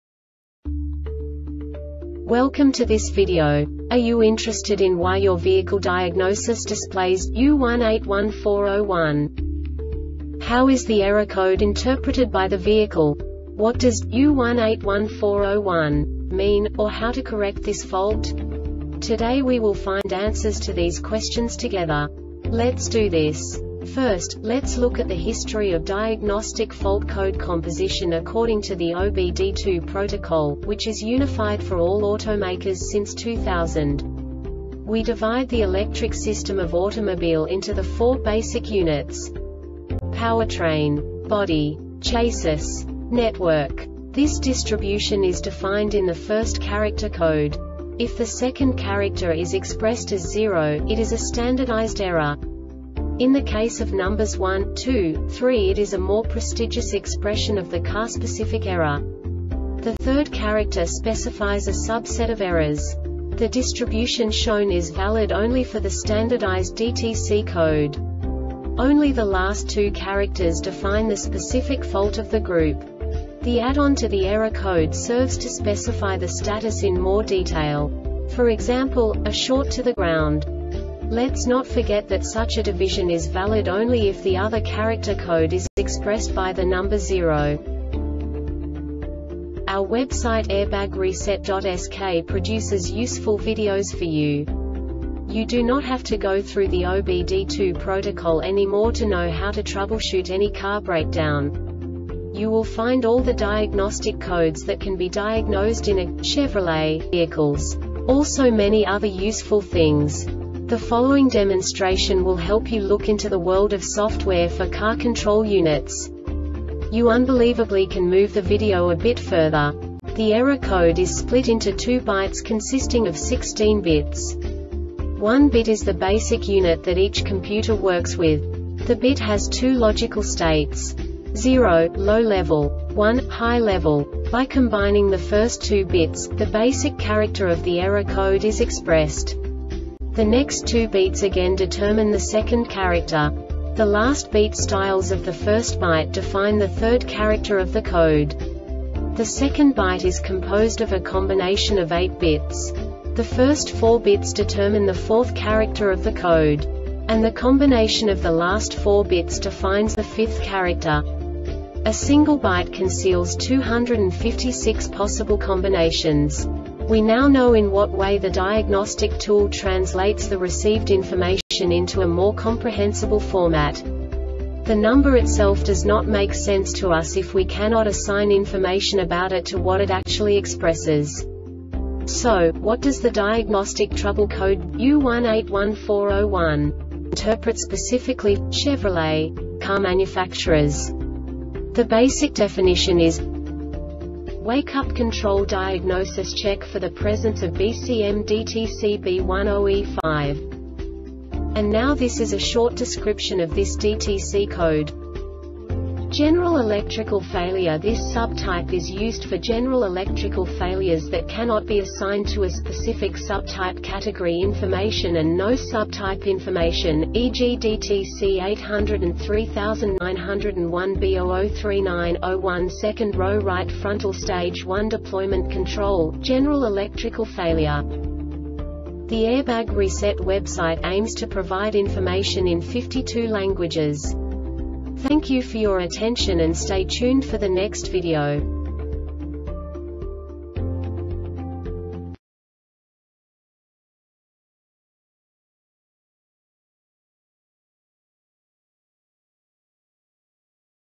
DTC Chevrolet U1814-01 Short Explanation
Contents:
0:21 Basic DTC analysis according to OBD2 protocol standard.
1:48 Insight into programming
2:58 A diagnostically understandable description of the Diagnostic Trouble Code
3:05 Basic error definition

Make:
Chevrolet
Code:
U1814 01
Definition:
Wakeup Control
Diagnosis:
Check for the presence of BCM DTC B10E5.